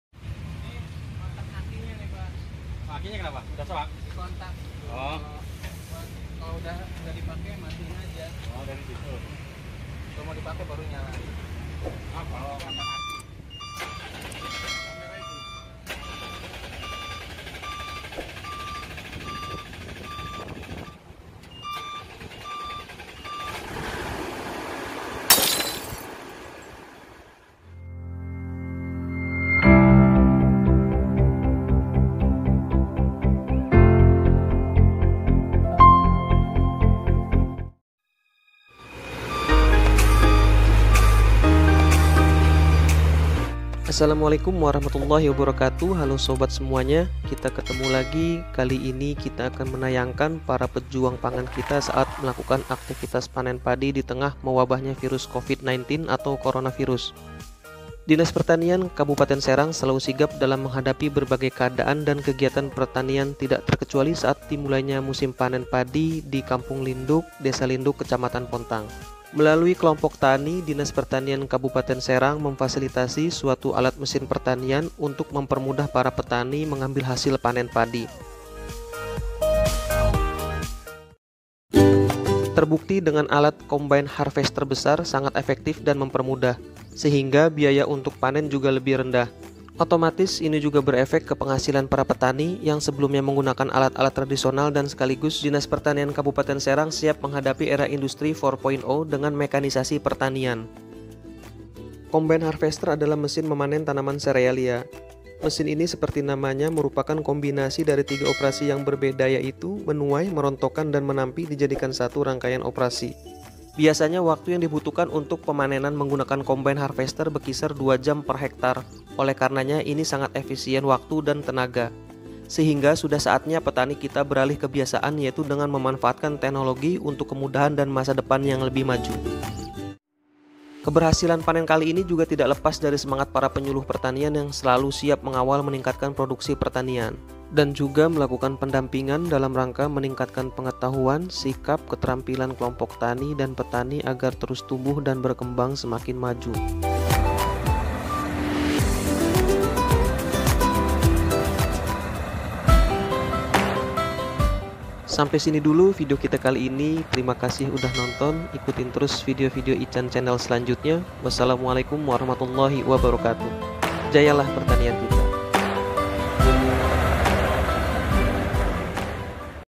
Panen Padi I Pakai Combine Harvester I panen padi ditengah wabah covid 19 I Breaking news
Ditengah mewabahnya virus corona muncul setitik harapan persembahan dari petani untuk Rakyat Indonesia...Saatnya petani kita bangkit dan maju dengan cara modern serta siap menghadapi tantangan industri 4.0...semoga tayangan ini memberikan semangat dan bermanfaat...mohon maaf apabila ada banyak sensor...🙏🙂